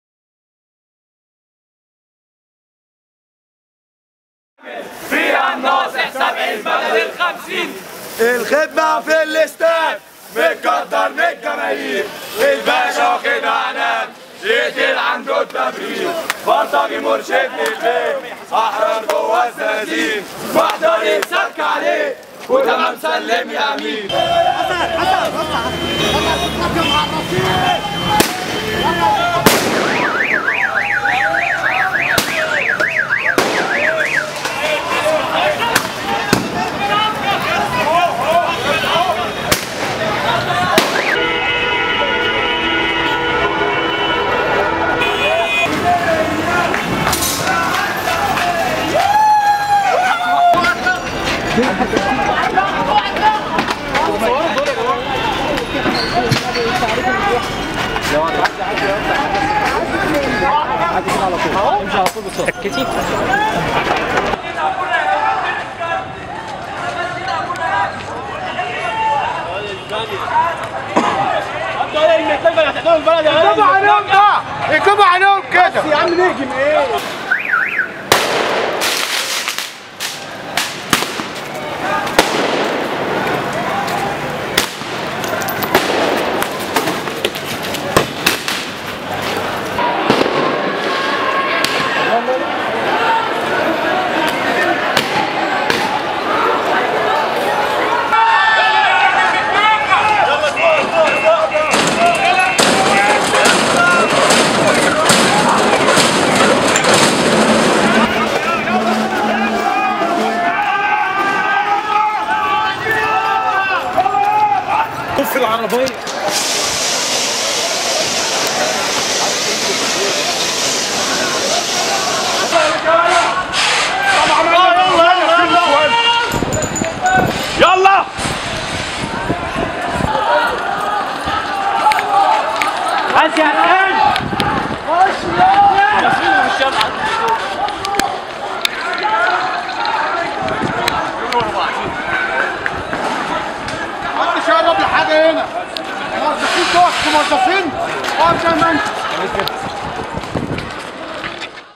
مجهولون يشتبكون مع طلاب كلية طب الفم والاسنان جامعة القاهرة
تصوير :- الحسيني محمد و سارة يونس و محمود حسن
مونتاج :- عبدالله ابوالغيط